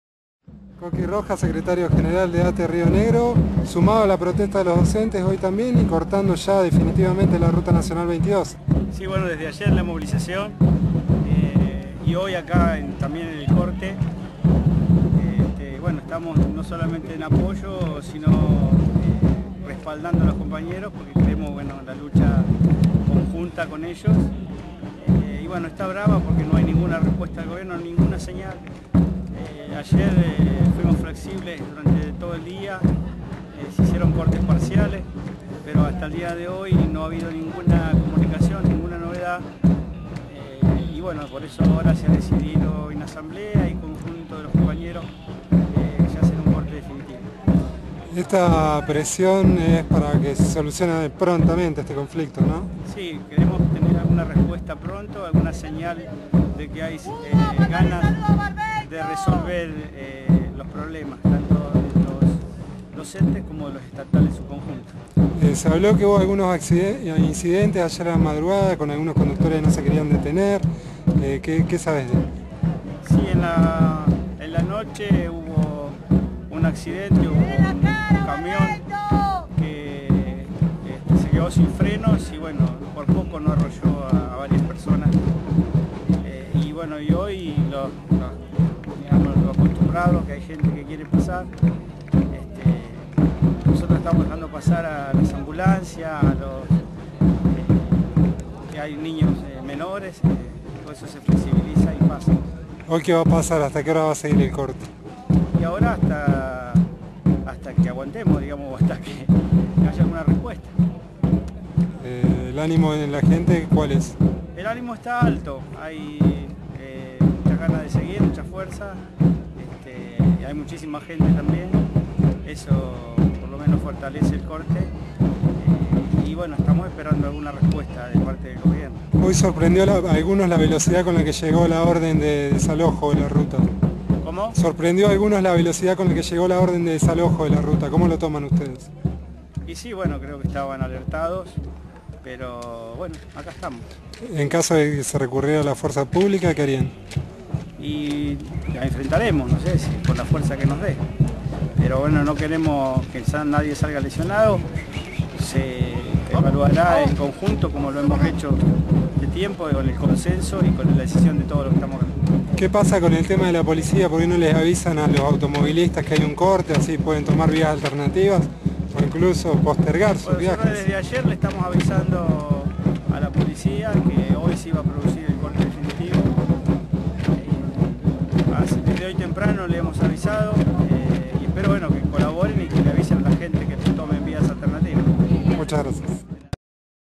ATE junto a la protesta docente en Río Negro
Hugo Rojas secretario general de ATE Río Negro ratificó el acompañamiento del gremio de estatales al reclamo salarial de los docentes rionegrinos que llevan adelante un paro por tiempo indeterminado y un corte de la ruta 22 a la altura de la localidad de Chichinales.